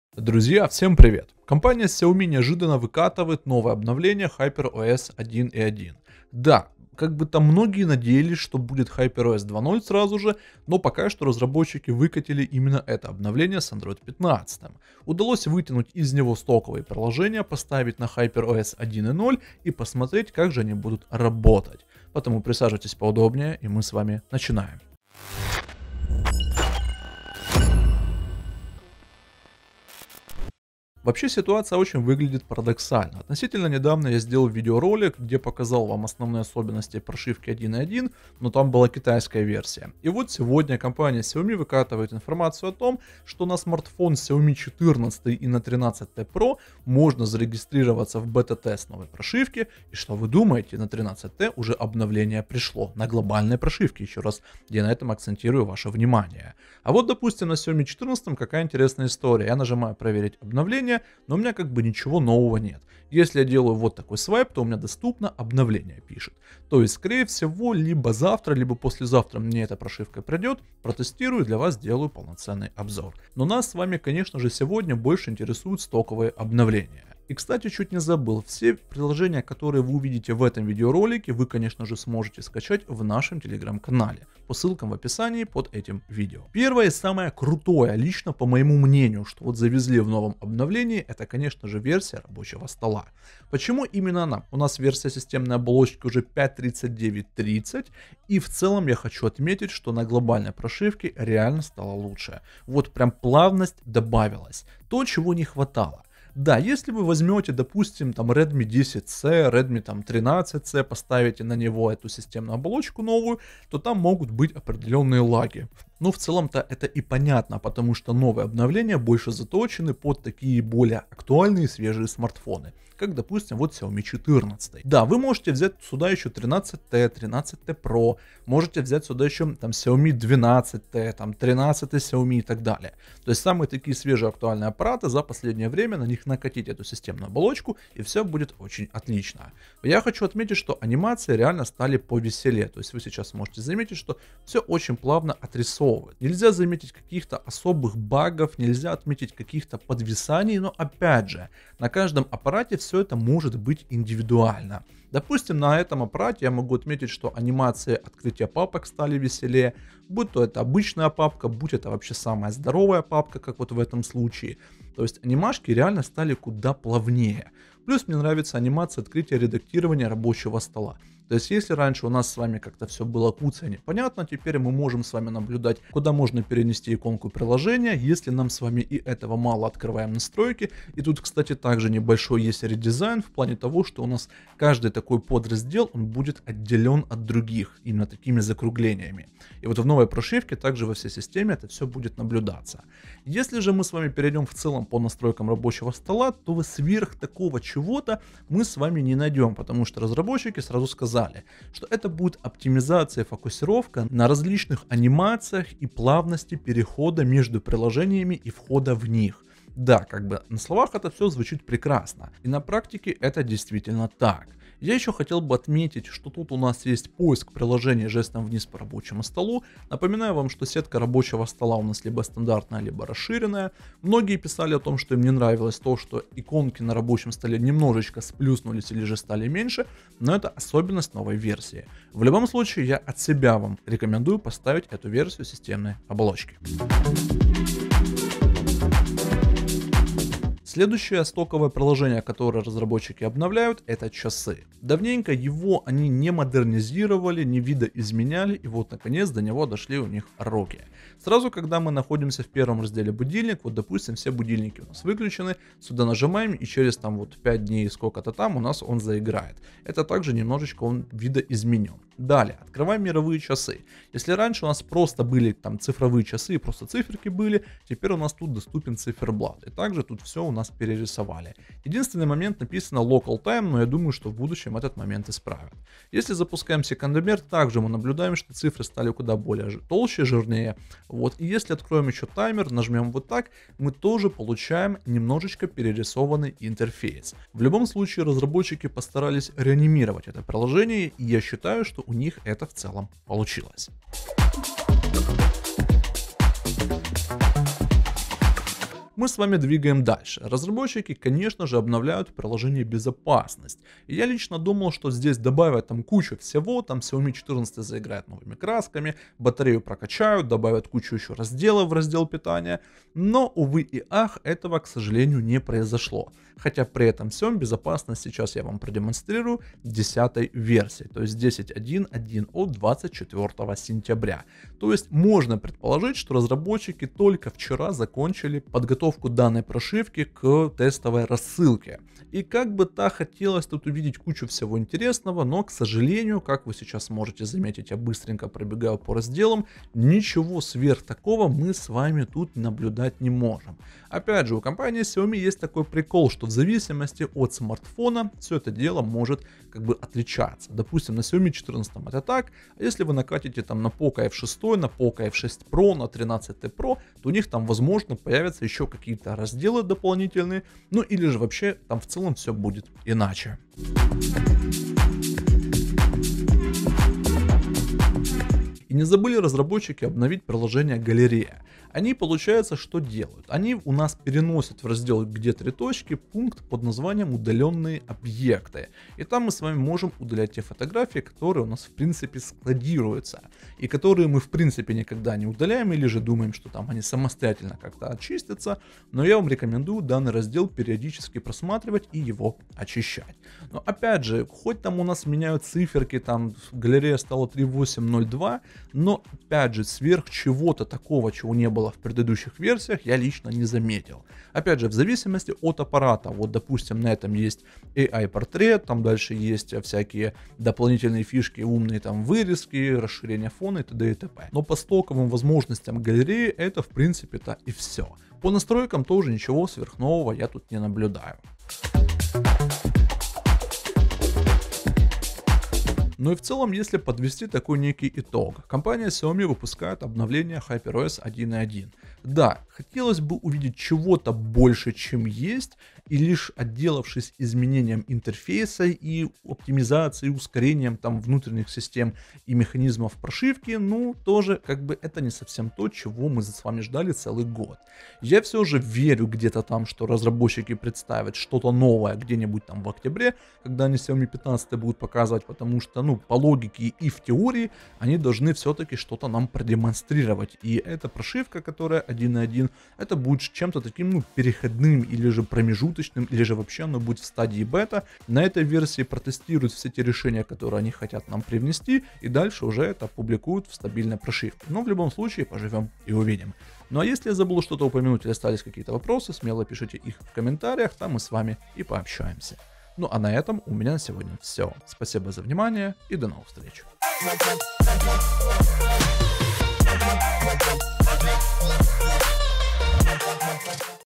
😲 Приложения из HyperOS 1.1 для твоего Xiaomi с HyperOS 1.0 и MIUI!
👉 Новые стоковые приложения из HyperOS 1.1 для твоего Xiaomi с HyperOS 1.0 и MIUI!